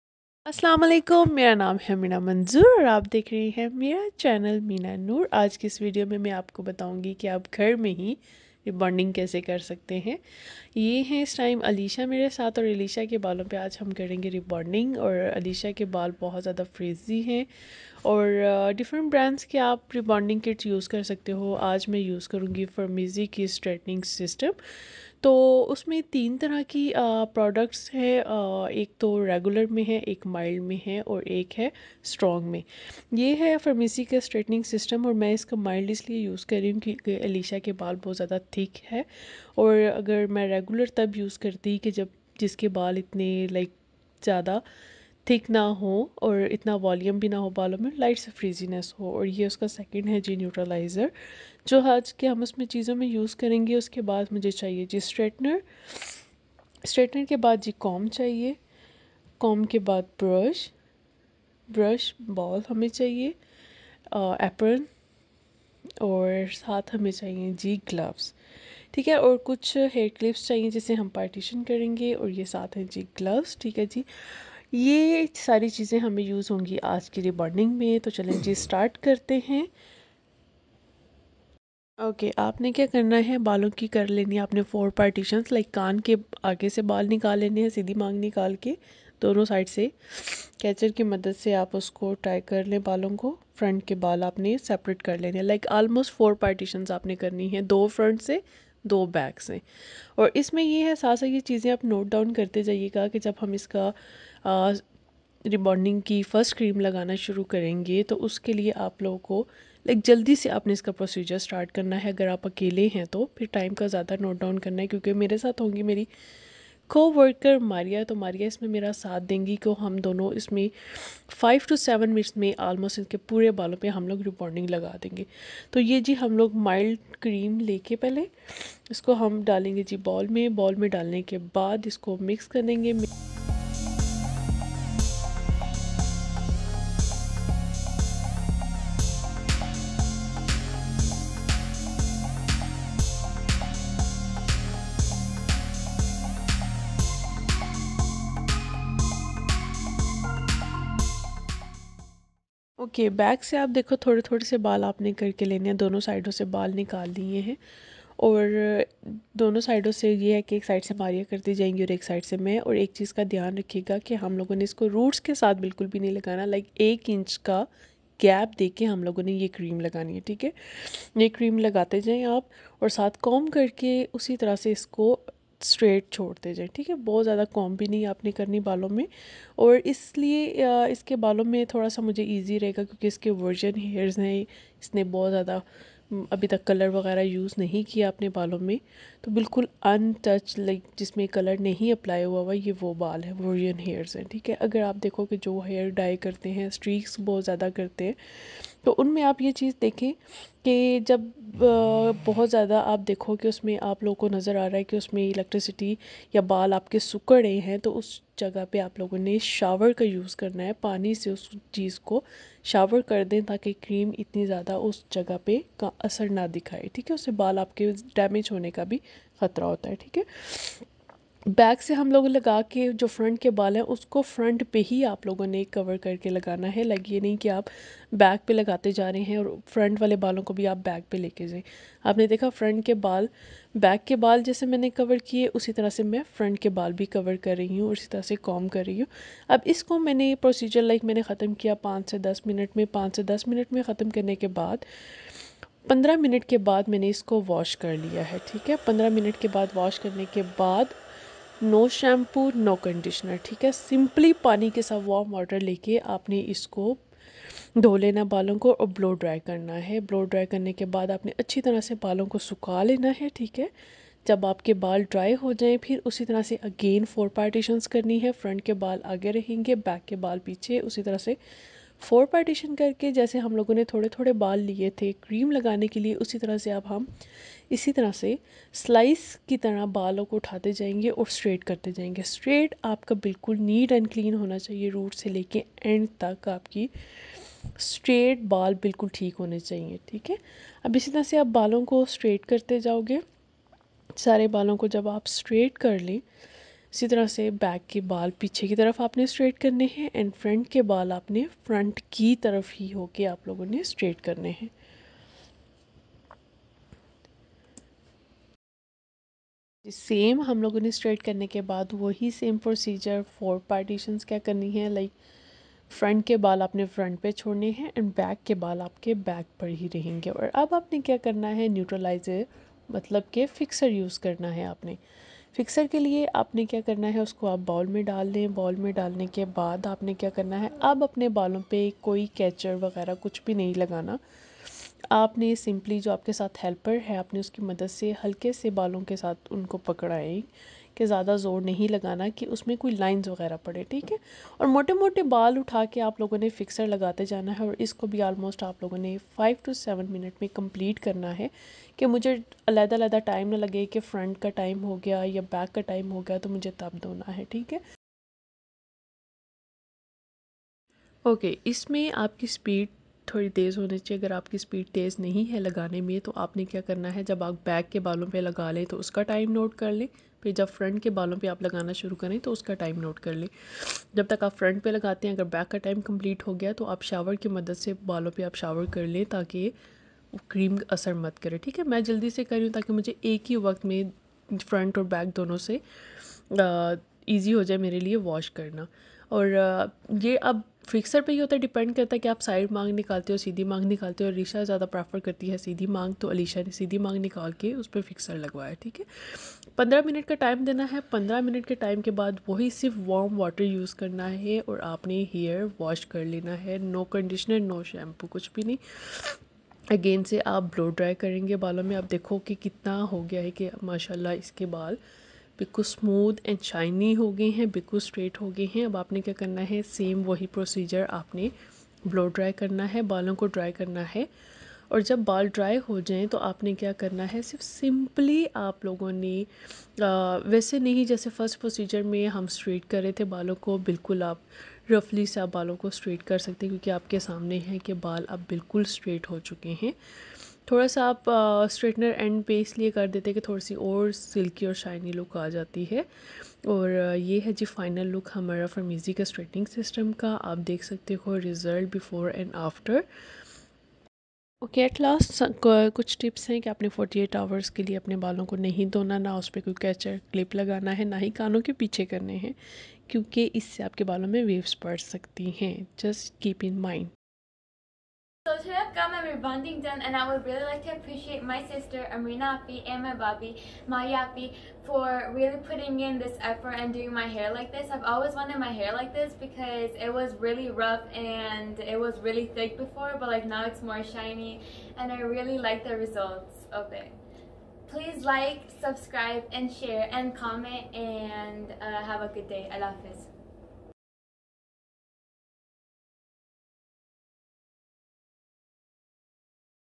How To Do Hair Rebounding At Home | Framesi Hair Straightening System| Step By Step In Urdu & Hindi
In this video I will be telling you on how to easily do hair rebounding at home. In today's video I have used Framesi' Hair Straightening Products. There are 3 products, regular, mild, and strong in this Framesi straightening system. Use regular if you have less frizzy hair, we use mild for thick hair, and we use strong if we have very curly hair. I definitely suggest this because it really gave the hair a huge glow and made it look so smooth. This is enough for today, keep tuned for more videos about hair, skin, etc.